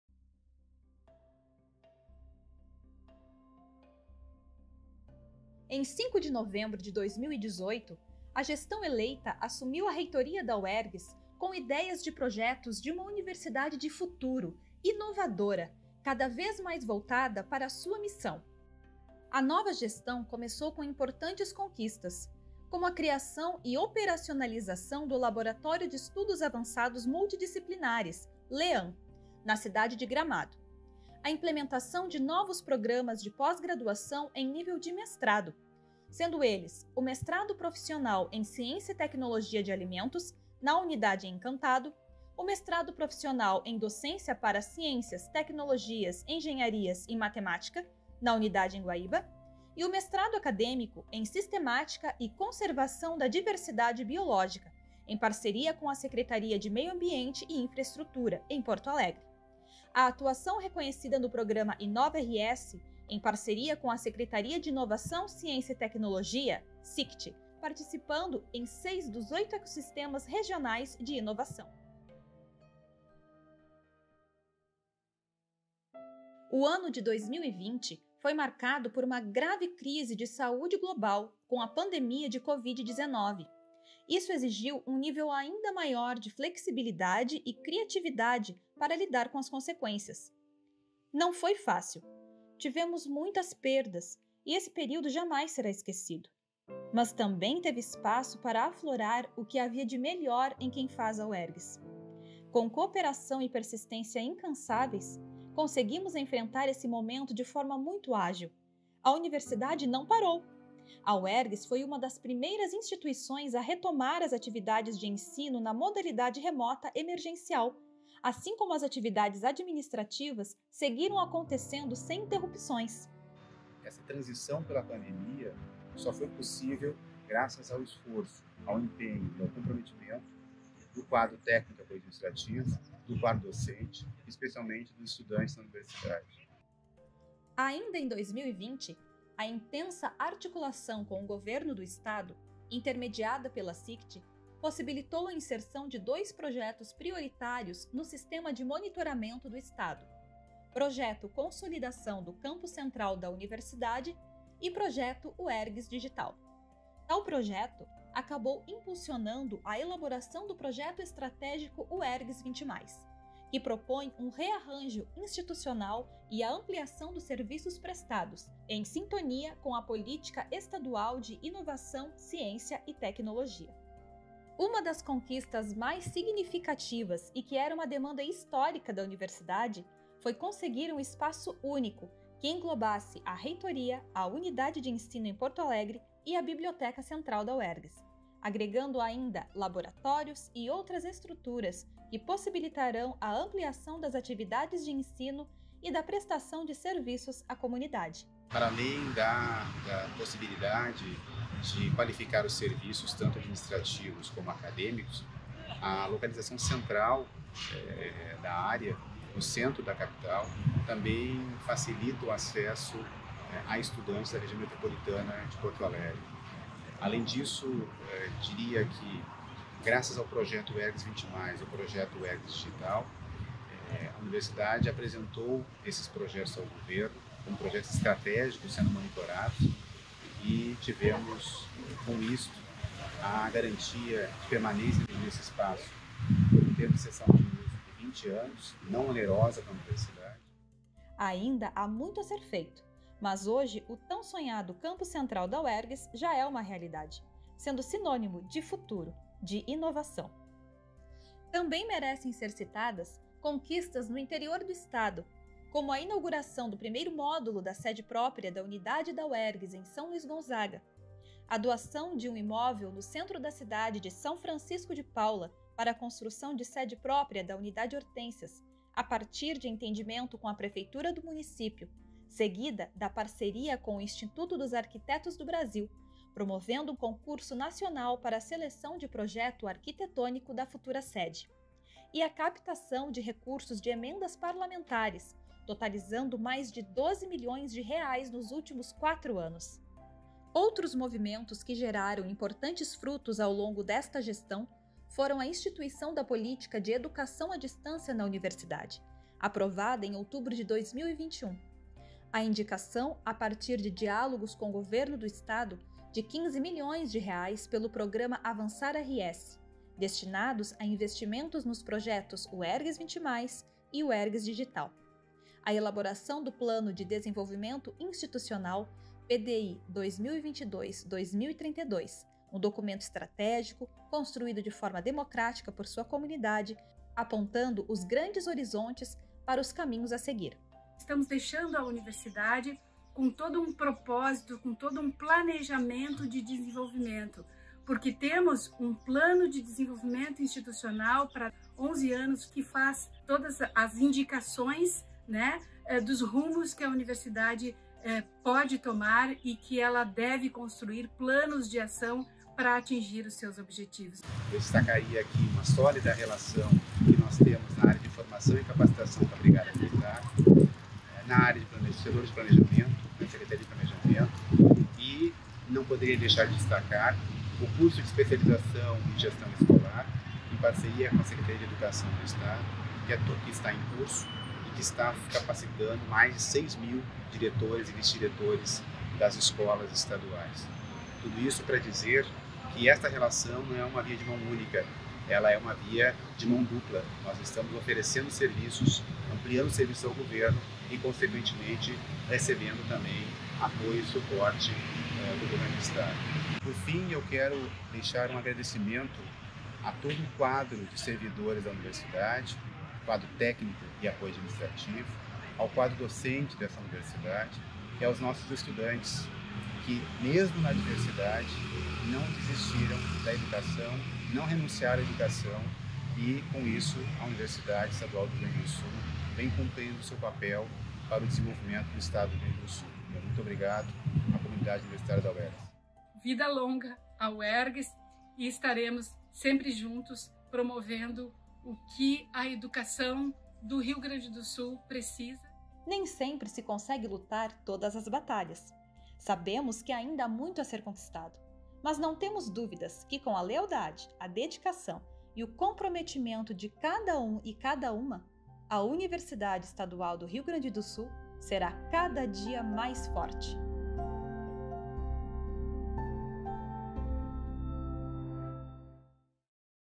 Destaques da Gestão 2018-2022
Vídeo que apresenta um resumo das principais conquistas que se destacaram na Gestão da Uergs para o período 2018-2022, do reitor Leonardo Beroldt e da vice-reitora Sandra Monteiro Lemos.